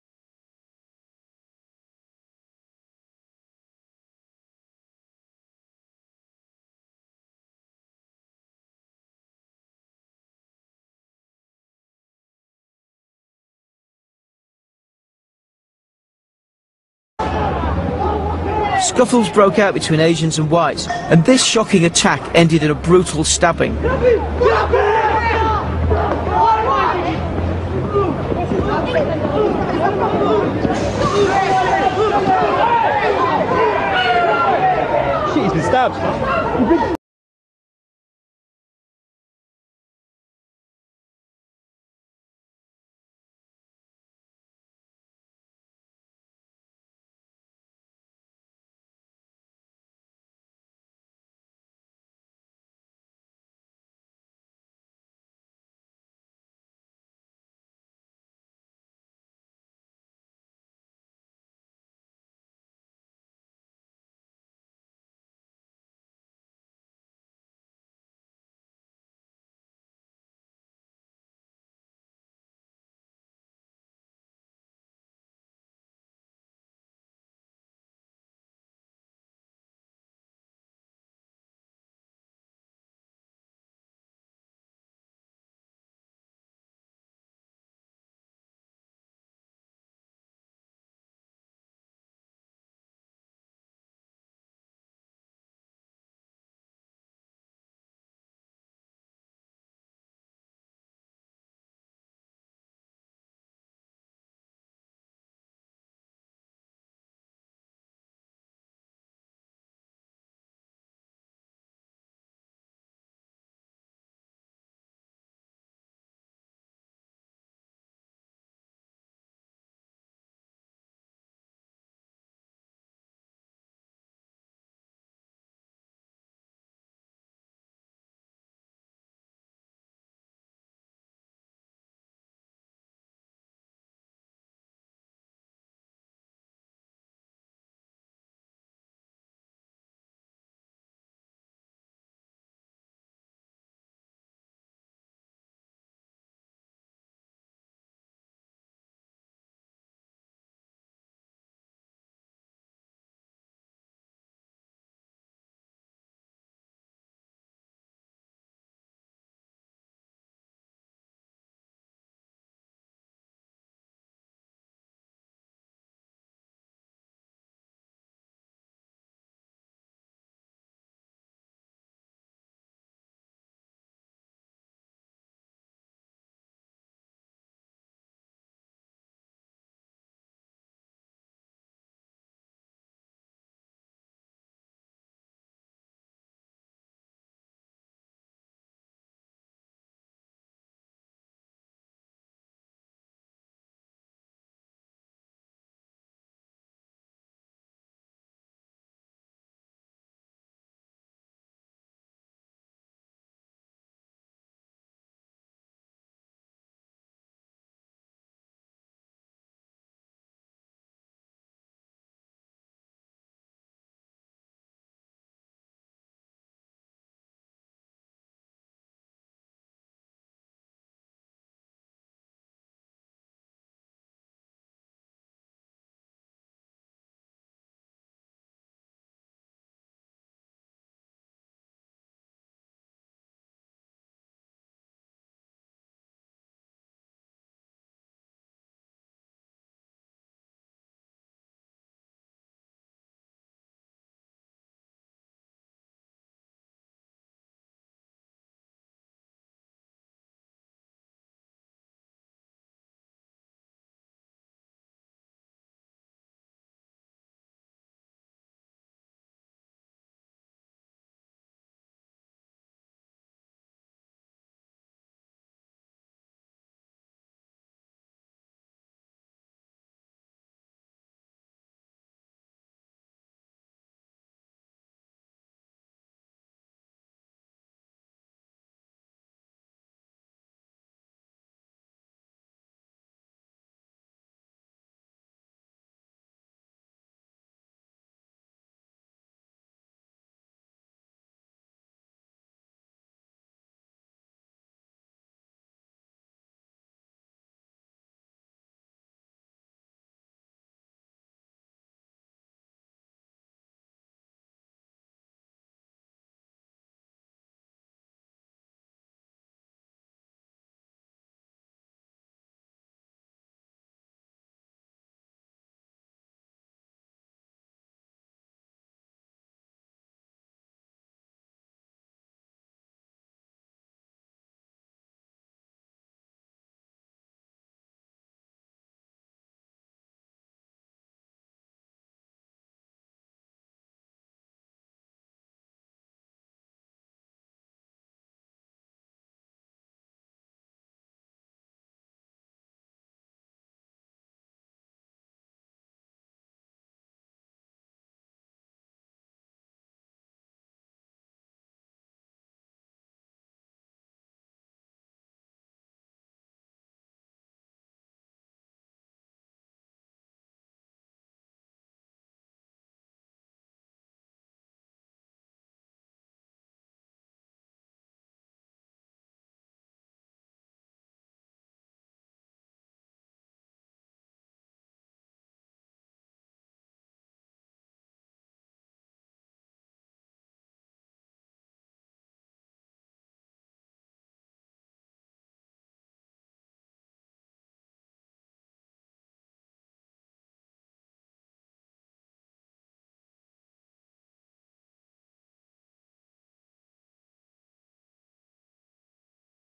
BRad Map Bradford Riots 2001 Research
A riot is one of the most intimate and visceral actions to take place in an urban environment. The action relates directly to built form, with the occupation and control of space being key in what is an outburst of tension between the polarities of what is likely a state of power and a class of inhabitants in that state. By using the Bradford riots of 2001 as a case study and texts such as Stephen Grahams: A New Military Urbanism, we hope to ask questions about the nature of control and occupation in our cities as well as to compile data and to create interactive visualisations to abstract our research. This film is a collection of some of the research undertaken.